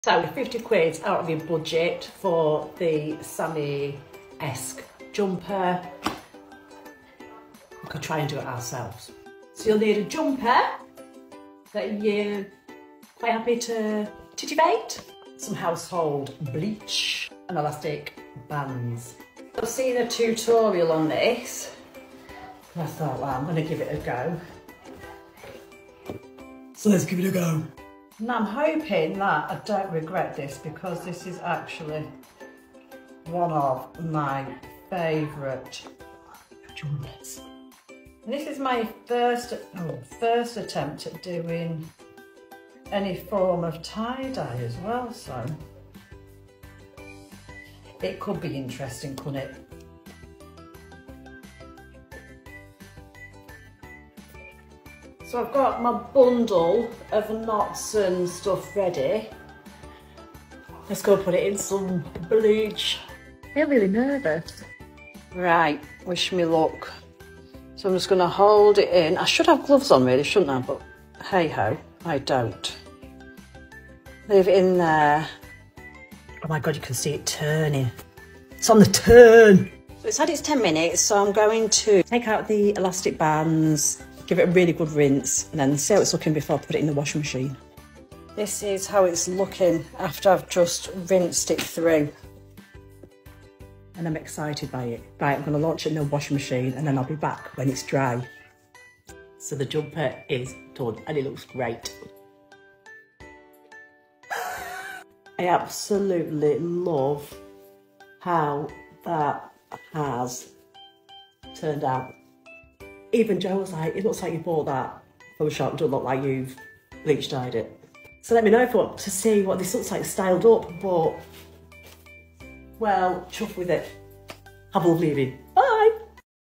Bleach dye transformation!
Bleach dying is my new favourite thing! Get a black jumper/ T-shirt, some house hold bleach, elastic bands.. and away you go!